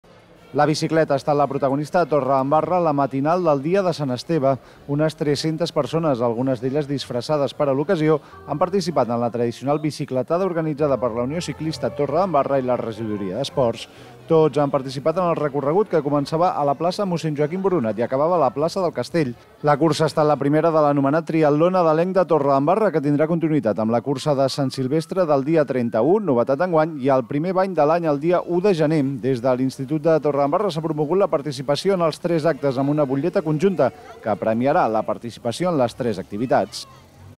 Torredembarra pedala per Sant Esteve
Unes 300 persones participen de la tradicional bicicletada popular. És la primera prova del Triatló nadalenc del municipi, que es completarà amb la cursa de Sant Silvestre i el primer bany de l'any.